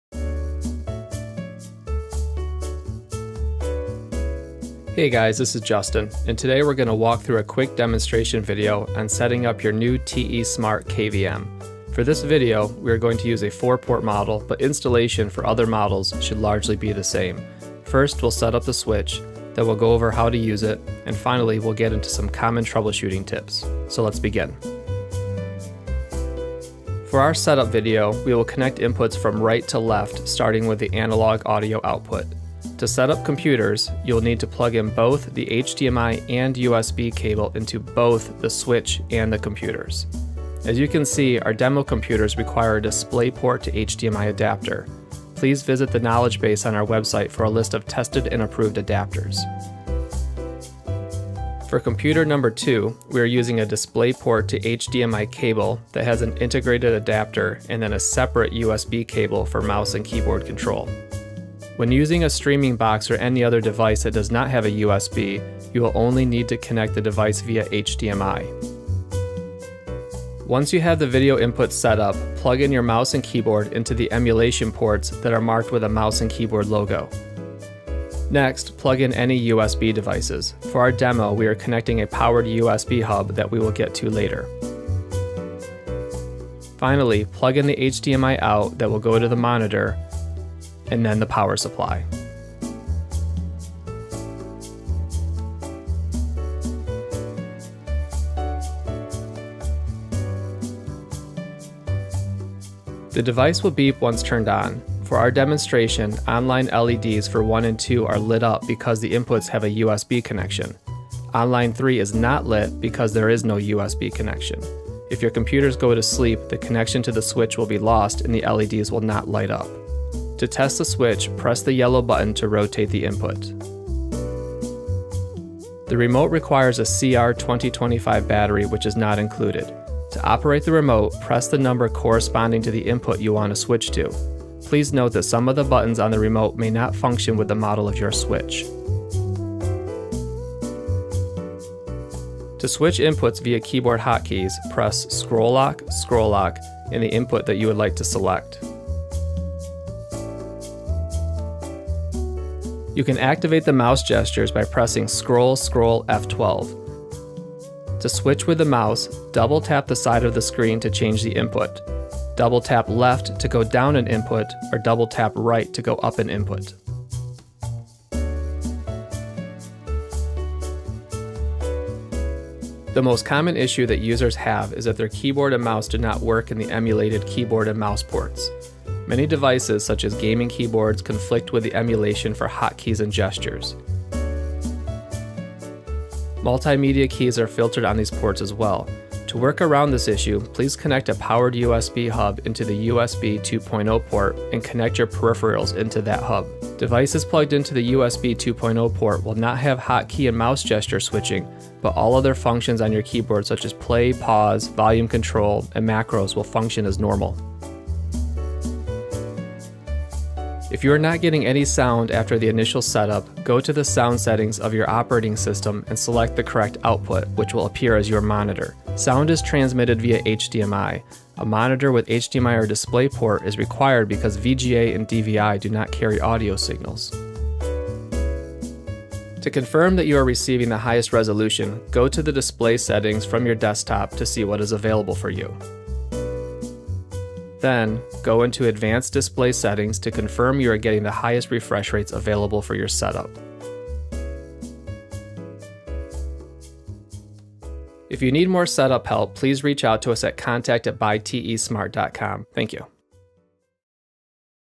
TESmart KVM Setup Video 4K 60hz HDMI KVM Switch
Featured KVM:
(4 computers-1 Monitor)

How to setup a KVM switch.  Today we are setting up a 4-port HDMI KVM Switch by TESmart.  These devices have HDMI 2.0 and achieve 4K@60hz.  Audio output and USB sharing.

TIMELINE
00:00 Introduction
00:25 Setting up the device
01:40 Operating the switch
02:19 Hotkeys
02:50 Troubleshooting
03:05 Mouse and keyboard
03:48 Checking the display
04:08 Conclusion

Corsair M65 Mouse
Sceptre u278w Monitor
Corsair K55 Keyboard
DisplayPort to HDMI Adapter (website adapter page)
Amazon Basics Mouse and Keyboard
Amazon Basics USB Hub
Bose SoundLink Mini Bluetooth Speaker